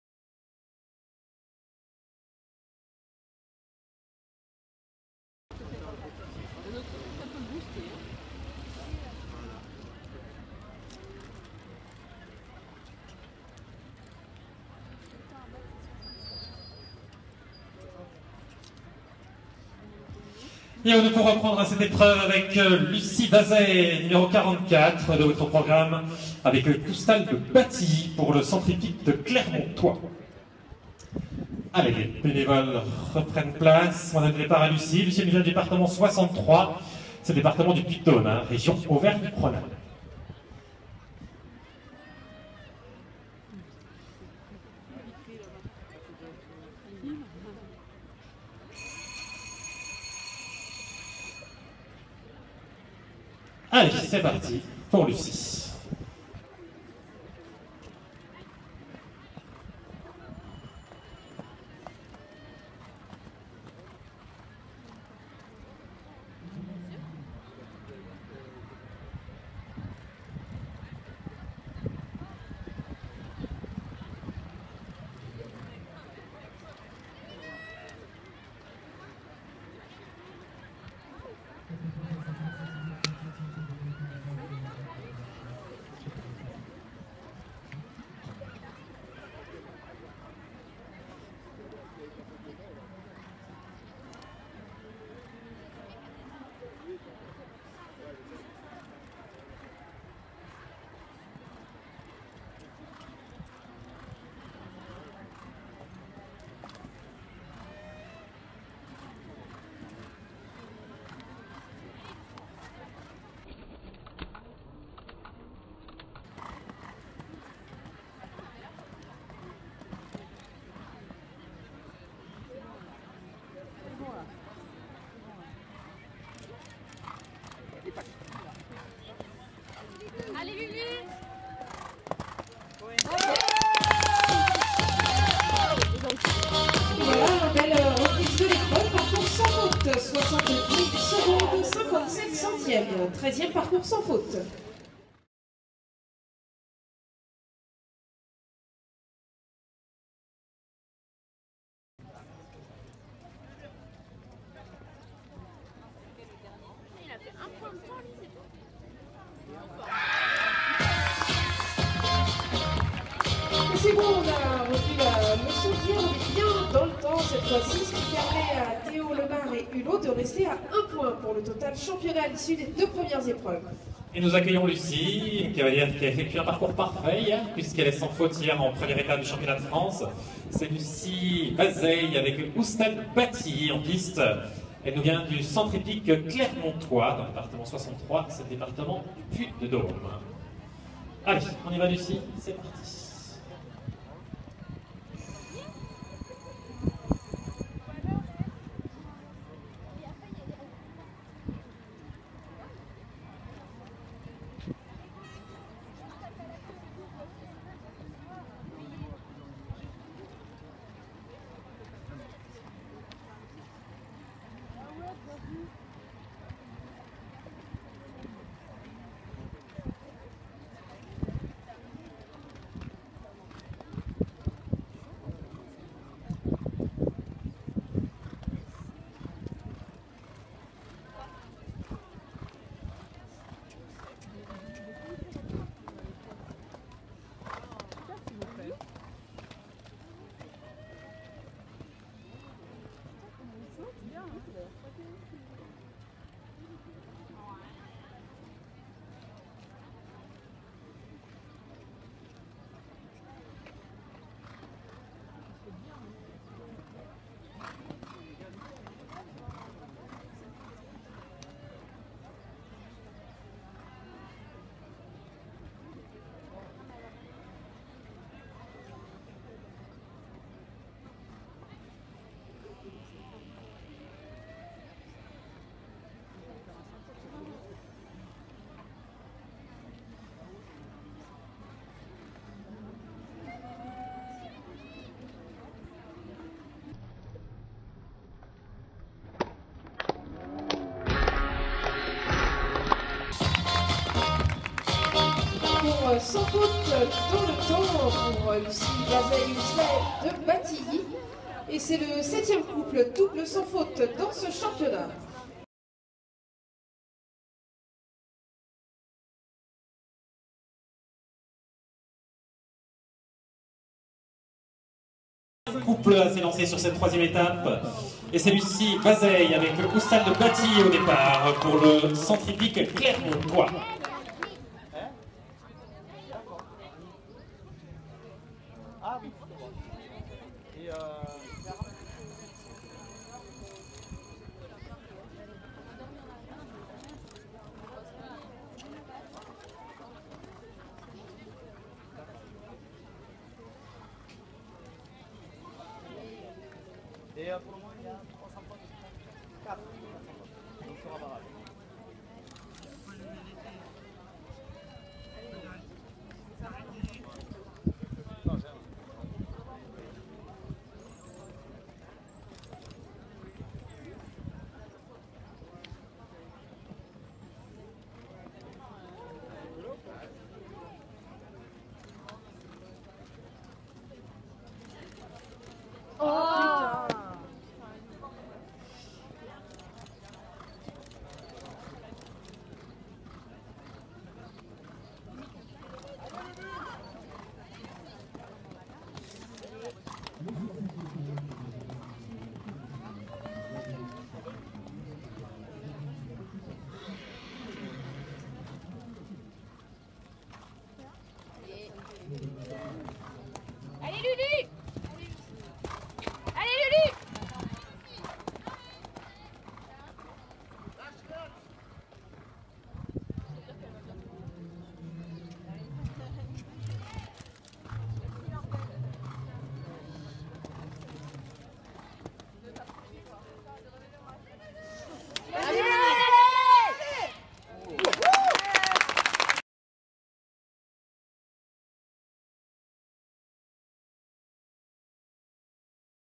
Open de France 2016 - Lucie Vazeille et Oustal de Batilly
Lucie et Oustal à l'Open de France Junior club élite excellence 2016.
Une belle performance : 12 ème sur 95 participants.